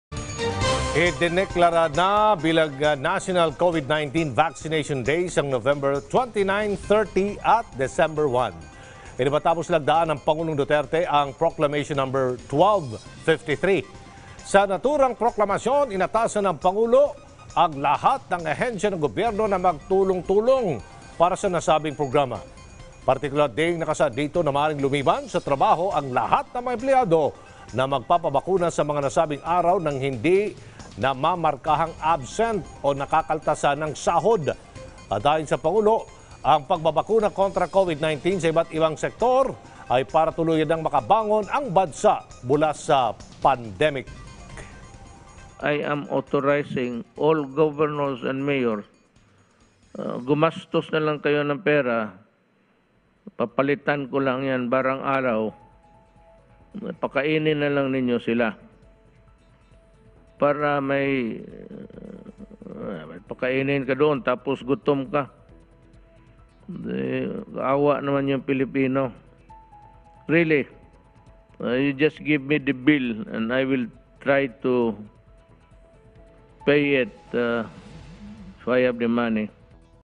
Pres. Duterte, idineklara na ang Nov. 29-Dec. 1 bilang National Vaccination Days
Pres. Duterte, idineklara na ang Nov. 29-Dec. 1 bilang National Vaccination Days; mga empleyadong magpapabakuna sa 3-day Nat’l Vaccination drive, ‘di makakaltasan ng sahod